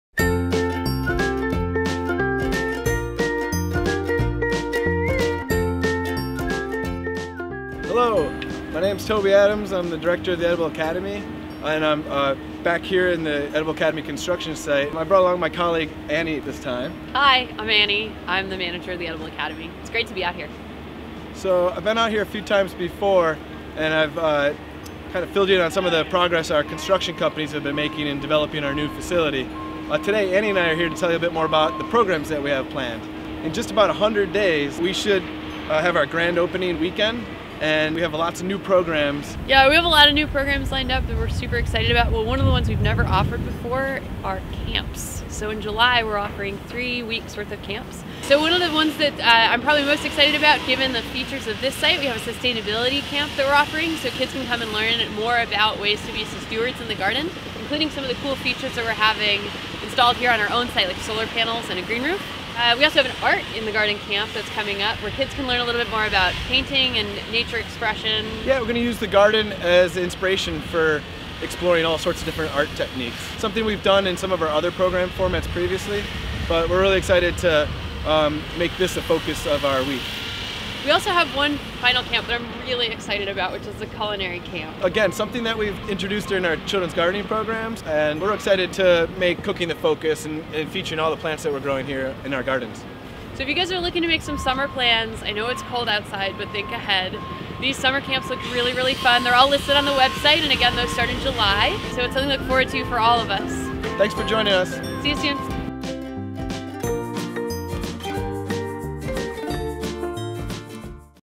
Making the Edible Academy: Programs & Events
We're back this week with an update on Making the Edible Academy, and progress is swift! Check in with Annie and Toby for a look into some of the programs for kids that we'll be offering when the Academy opens in June.